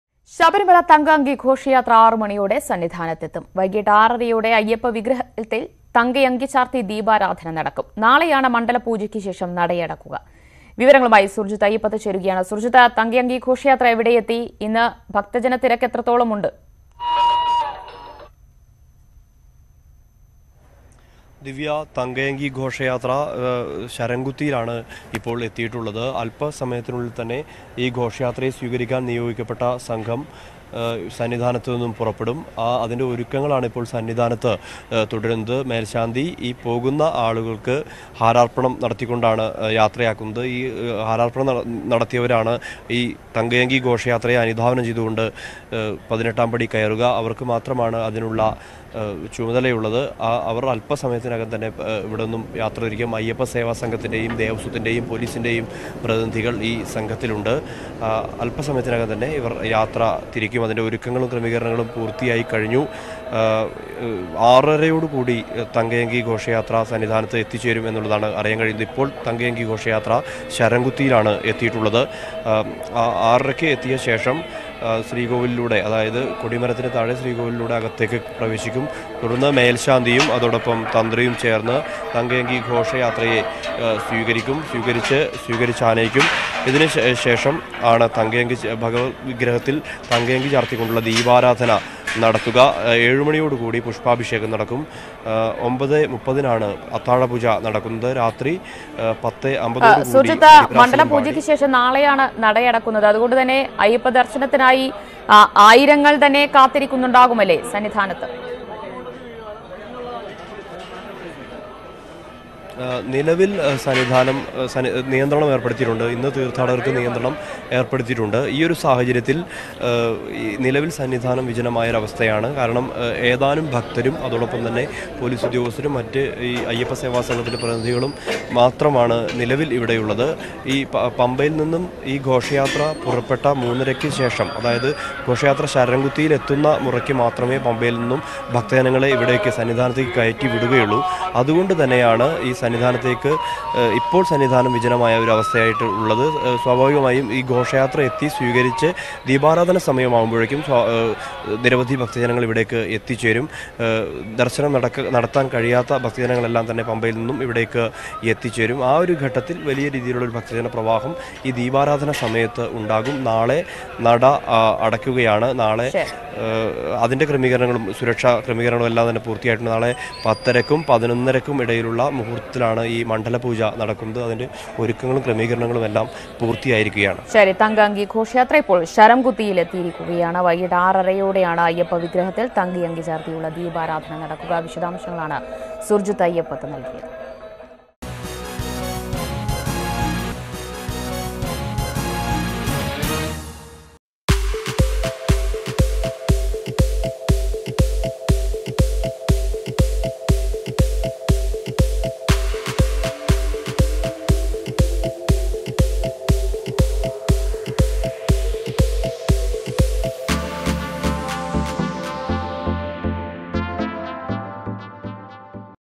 Sabarimala Live: ശബരിമല തങ്കയങ്കി ഘോഷയാത്ര അല്പ സമയത്തിനകം സന്നിധാനത്ത് എത്തും
The Thankayanki Ghoshayathra, the procession carrying the sacred golden dress of Lord Ayyappa to reach Sabarimala Sannidhanam in few hours. This will mark the culmination of 41-day-long Mandalakalam pilgrimage.